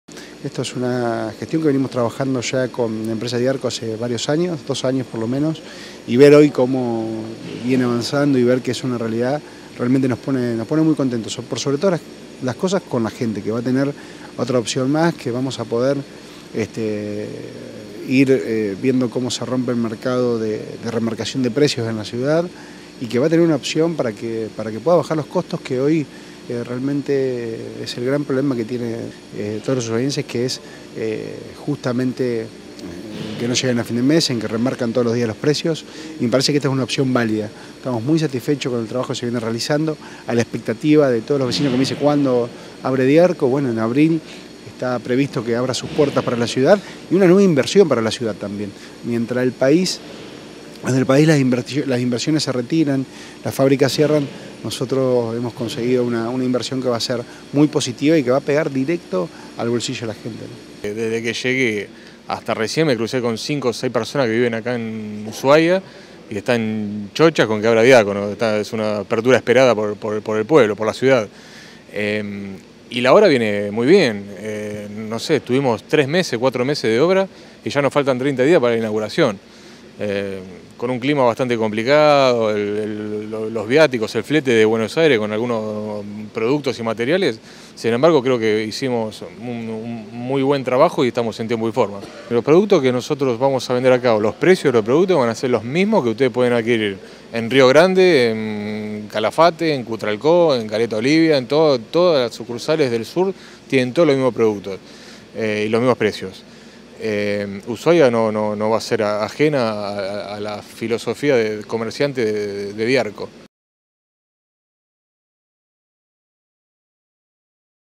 Ushuaia: Se confirmó la llegada de DIARCO
“Tenemos muchas expectativas, logramos que se invierta en la ciudad, se genere trabajo y se beneficie a los vecinos”, afirmaron.